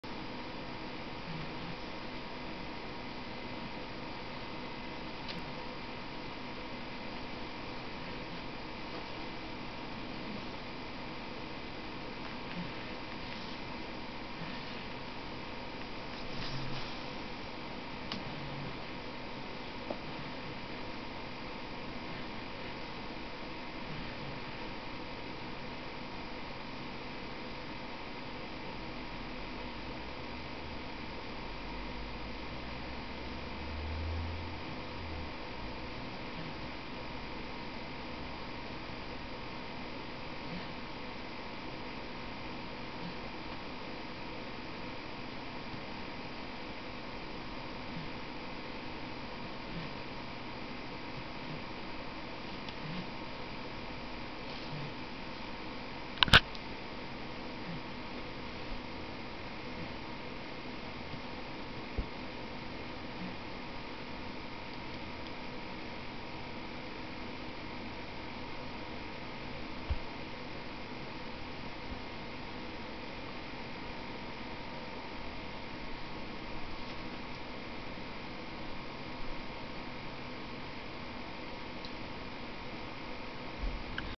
Dog dreaming
Older dog dreaming (probably about the ball they've just destroyed) with the younger one looking a little put out! Was very vocal just before the camera was ready...typical!